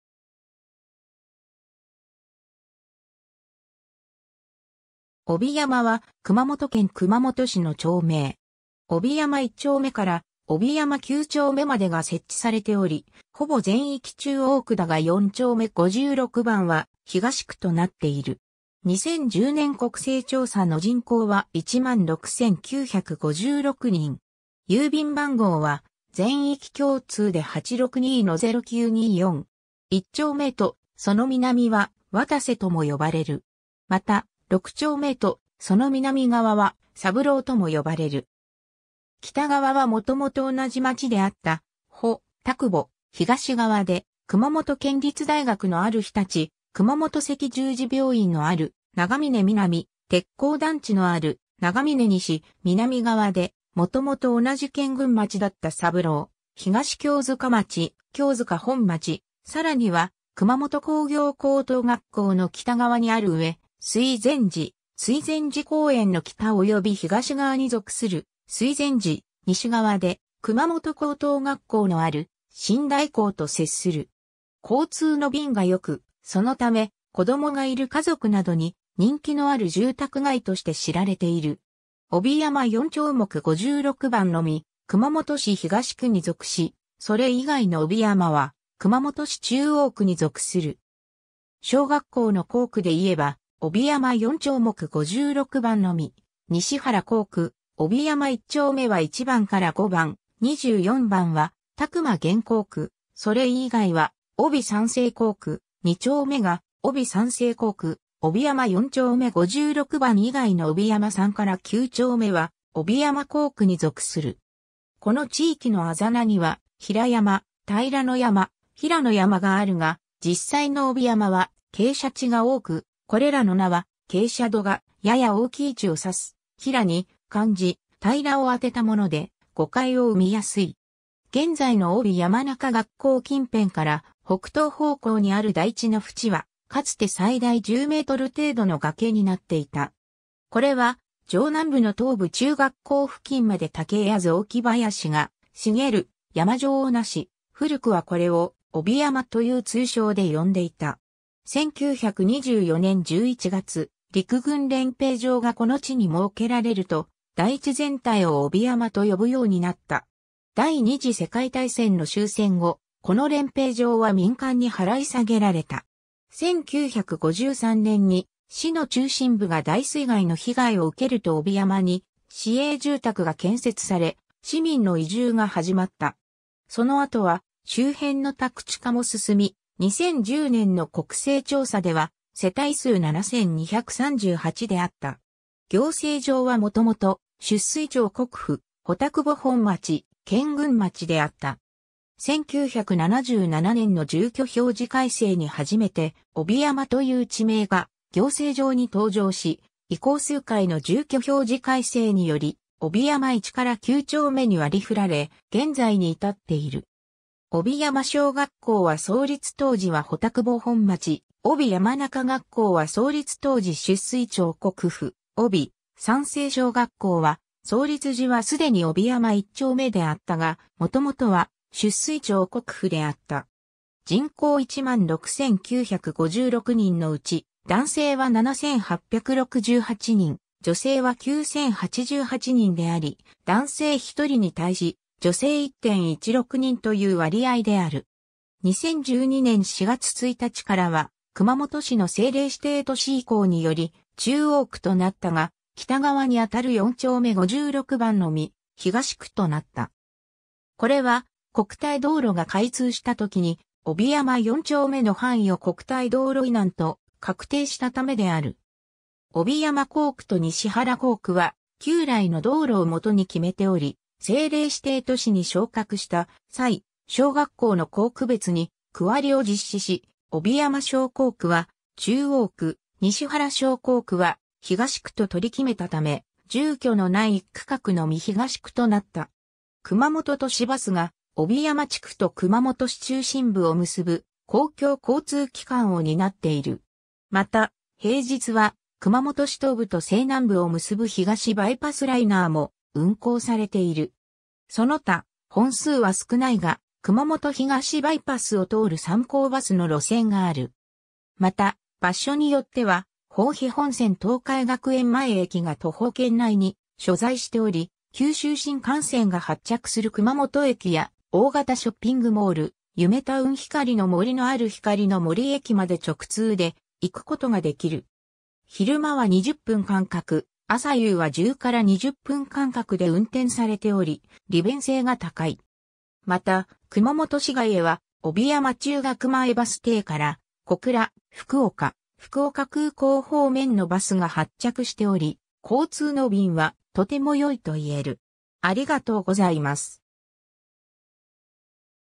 帯山
熊本市の町・字
帯山（おびやま）は、熊本県熊本市の町名。
帯山一丁目から帯山九丁目までが設置されており、ほぼ全域中央区だが四丁目56番は東区となっている。
2010年国勢調査の人口は16,956人。
（すべて中央区の人口・東区は0人）郵便番号は全域共通で862-0924。
 一丁目と、その南は渡瀬（わたるぜ）とも呼ばれる。
また、六丁目と、その南側は三郎（さぶろう）とも呼ばれる。
北側はもともと同じ町であった保田窪、東側で熊本県立大学のある月出、熊本赤十字病院のある長嶺南、鉄工団地のある長嶺西、南側で、もともと同じ健軍町だった三郎・東京塚町・京塚本町、さらには熊本工業高等学校の北側にある上水前寺、水前寺公園の北及び東側に属する水前寺、西側で熊本高等学校のある新大江と接する。
 交通の便がよく（熊本都市バス、産交バス等が頻繁に走っている）、そのため、子供がいる家族などに人気のある住宅街として知られている。
 帯山四丁目56番のみ熊本市東区に属し、それ以外の帯山は熊本市中央区に属する。
小学校の校区で言えば、帯山四丁目56番のみ西原（にしばる）校区、帯山一丁目は1番から5番、24番は託麻原校区（4番20号から34号、24番は帯山西小学校の選択も可能）、それ以外は帯山西校区、二丁目が帯山西校区、帯山四丁目56番以外の帯山三～九丁目は帯山校区に属する。
 この地域の字名には「平山」「平ノ山」「平野山」があるが実際の帯山は傾斜地が多く、これらの名は傾斜度がやや大きい地を指す「ヒラ」に漢字「平」を当てたもので、誤解を生みやすい。
 現在の帯山中学校近辺から北東方向にある台地の淵は、かつて最大10m程度の崖になっていた。
これは、上南部の東部中学校付近まで竹や雑木林が茂る山状を成し、古くはこれを「帯山」という通称で呼んでいた。
1924年（大正13年）11月、陸軍練兵場がこの地に設けられると、台地全体を帯山と呼ぶようになった。
 第二次世界大戦の終戦後、この練兵場は民間に払い下げられた。
1953年（昭和28年）に市の中心部が大水害の被害を受けると帯山に市営住宅が建設され、市民の移住が始まった。
その後は周辺の宅地化も進み、2010年の国勢調査では世帯数7,238であった。
 行政上はもともと、出水町国府（帯山一丁目～三丁目）、保田窪本町（帯山二・四～九丁目）、健軍町（帯山三丁目・六丁目）であった。
1977年（昭和54年）の住居表示改正に初めて帯山という地名が行政上に登場し、以降数回の住居表示改正により、帯山一～九丁目に割り振られ、現在に至っている。
帯山小学校は創立当時は保田窪本町、帯山中学校は創立当時出水町国府、帯山西小学校は創立時はすでに帯山一丁目であったが、もともとは出水町国府であった。
 人口16,956人のうち、男性は7,868人、女性は9,088人であり、男性１人に対し、女性1.16人という割合である。
 2012年4月1日からは、熊本市の政令指定都市移行により中央区となったが、北側にあたる四丁目56番のみ東区となった。
（店舗・事務所のみで、住居は1軒もない） これは、国体道路が開通したときに、帯山四丁目の範囲を国体道路以南と確定したためである。
帯山校区と西原（にしばる）校区は、旧来の道路（国体道路より１本南側の市道）を基に決めており、政令指定都市に昇格した際、小学校の校区別に区割りを実施し、帯山小校区は中央区、西原小校区は東区と取り決めたため、住居のない１区画のみ東区となった。
(もともとは住居もあった) 熊本都市バスが、帯山地区と熊本市中心部を結ぶ公共交通機関を担っている。
また、平日は熊本市東部と西南部を結ぶ東バイパスライナーも運行されている。
その他、本数は少ないが熊本東バイパスを通る産交バスの路線がある。
また、場所によっては豊肥本線東海学園前駅が徒歩圏内に所在しており、九州新幹線が発着する熊本駅や、大型ショッピングモール｢ゆめタウン光の森｣のある光の森駅まで直通で行くことが出来る。
昼間は20分間隔、朝夕は10-20分間隔で運転されており、利便性が高い。
また、熊本市外へは、帯山中学前バス停から、小倉、福岡、福岡空港方面のバスが発着しており、交通の便はとても良いと言える。